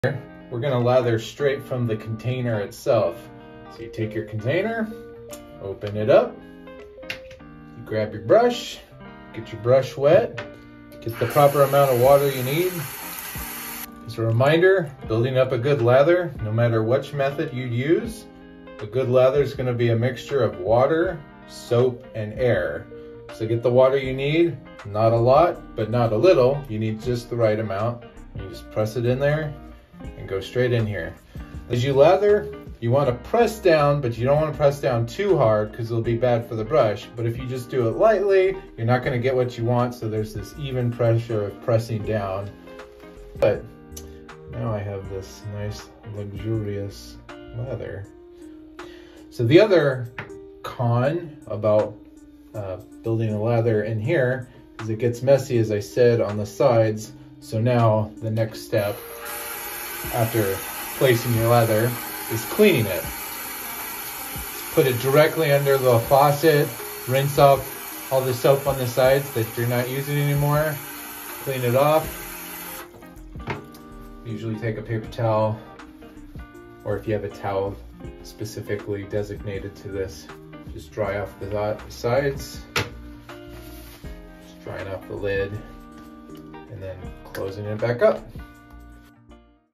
How to Lather Straight From the Container
Elevate your grooming game with this convenient and hassle-free lathering method. Watch as the rich, creamy lather effortlessly forms, providing you with a smooth and comfortable shave.